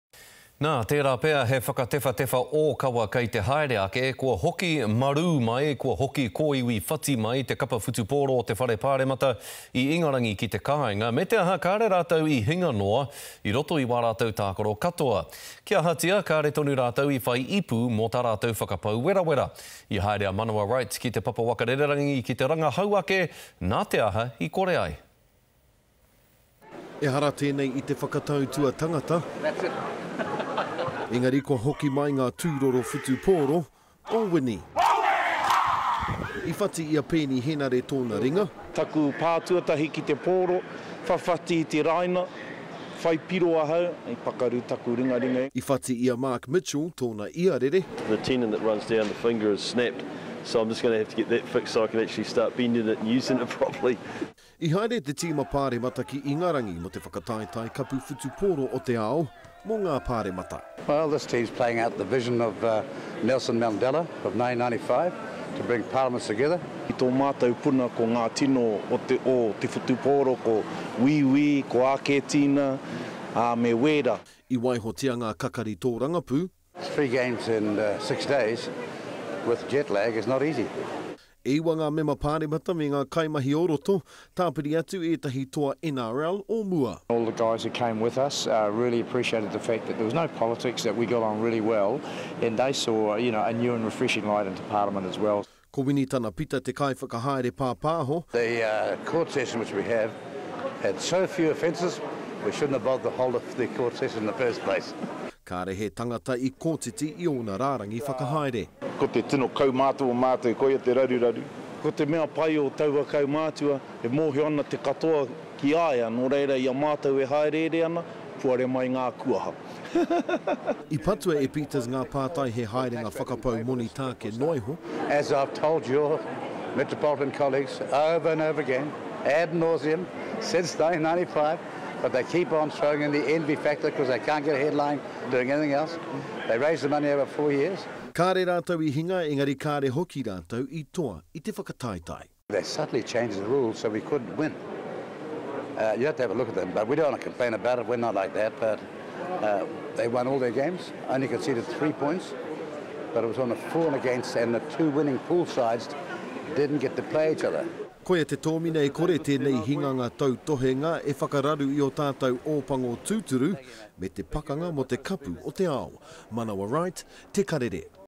Undefeated MPs' rugby team returns battered, bruised...and empty-handed?
There might be an official inquiry. The NZ Parliamentary Rugby Team returned from England bruised, broken, but undefeated, yet they came home empty-handed. Manawa Wright went out to the airport to investigate.